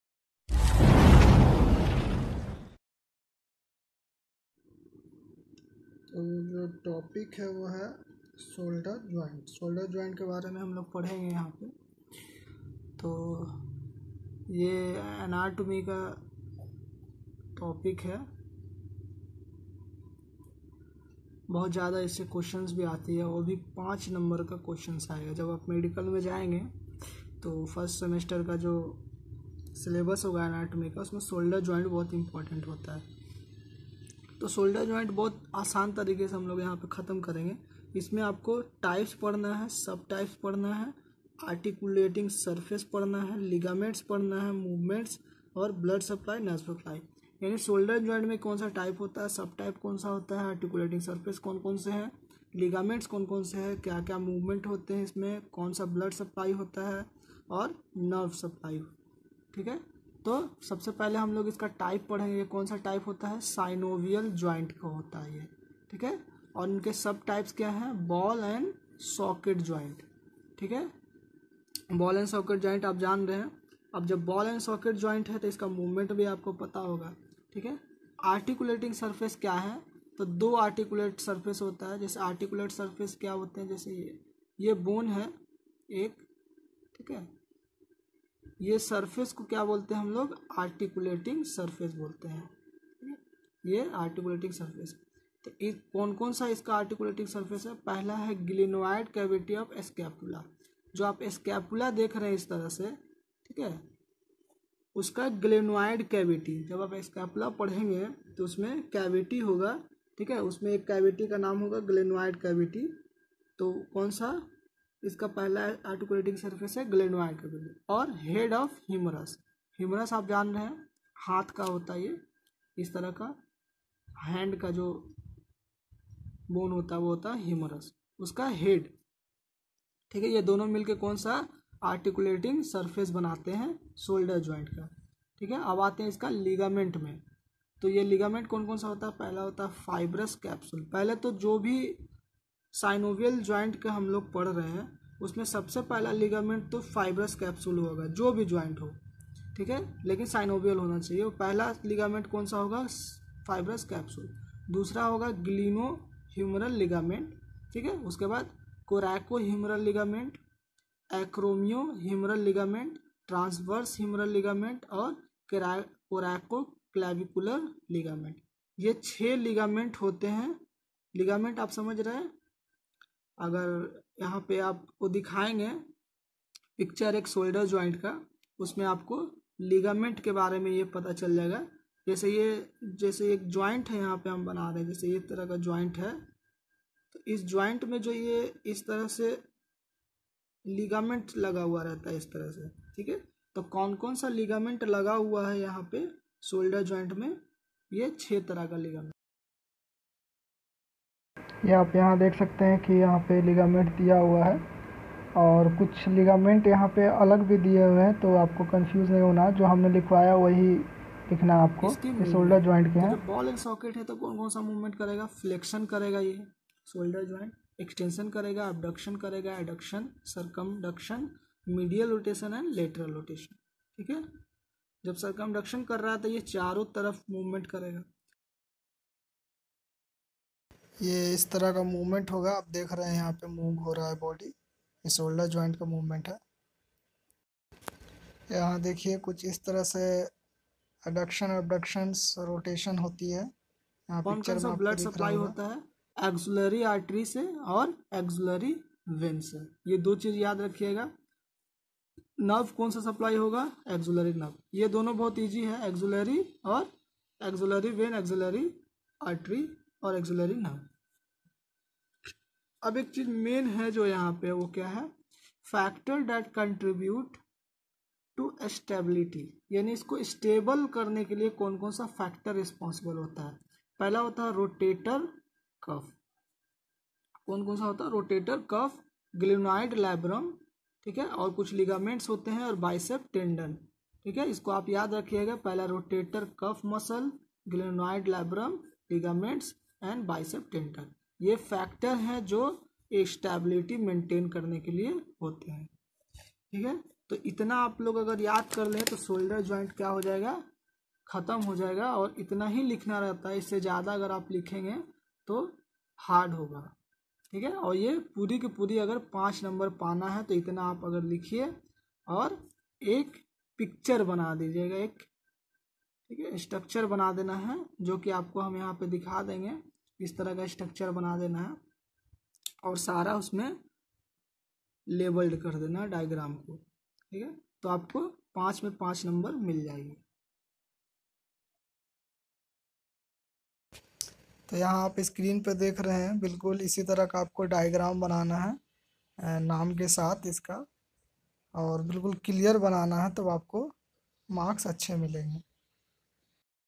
Shoulder joint anatomy detail in hindi in easy way
Shoulder joint
Type
Subtype
Ligament
Movements
Articulating surface

It extends from the anatomical neck of the humerus to the border or ‘rim’ of the glenoid fossa. The joint capsule is lax, permitting greater mobility (particularly abduction).

The synovial membrane lines the inner surface of the joint capsule, and produces synovial fluid to reduce friction between the articular surfaces.

To reduce friction in the shoulder joint, several synovial bursae are present. A bursa is a synovial fluid filled sac, which acts as a cushion between tendons and other joint structures.

The bursae that are important clinically are:

Subacromial – located deep to the deltoid and acromion, and superficial to the supraspinatus tendon and joint capsule. The subacromial bursa reduces friction beneath the deltoid, promoting free motion of the rotator cuff tendons. Subacromial bursitis (i.e. inflammation of the bursa) can be a cause of shoulder pain.
Subscapular – located between the subscapularis tendon and the scapula. It reduces wear and tear on the tendon during movement at the shoulder joint.
There are other minor bursae present between the tendons of the muscles around the joint, but this is beyond the scope of this article.